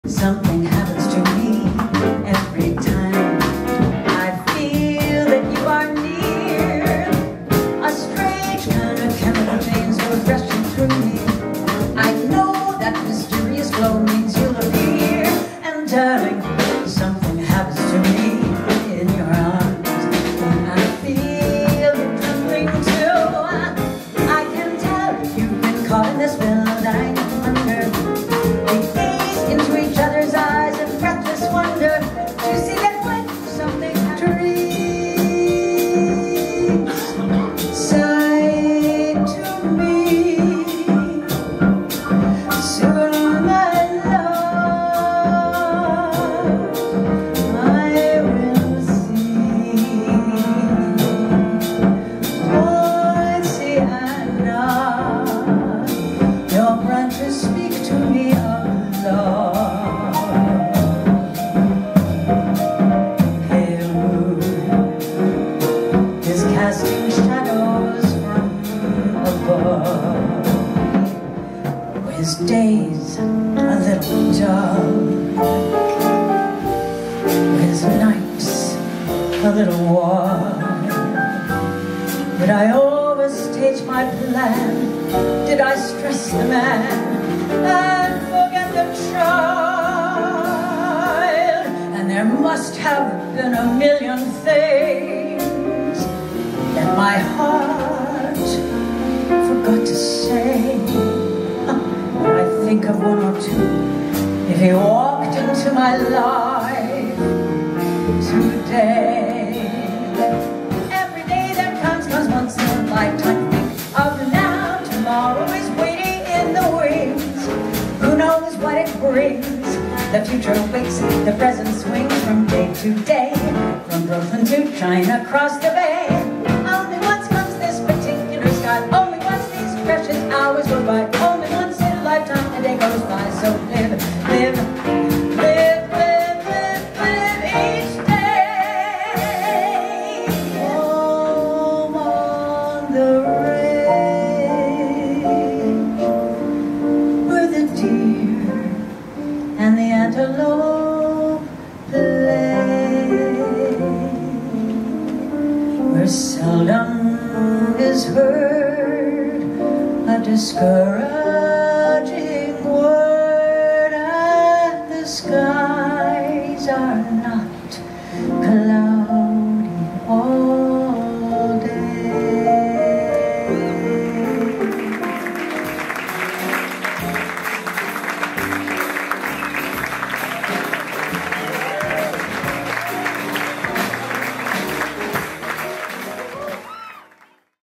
Linda Purl at 54 Below
Highlights from Linda's performance at 54 Below in New York City on October 22, 2024.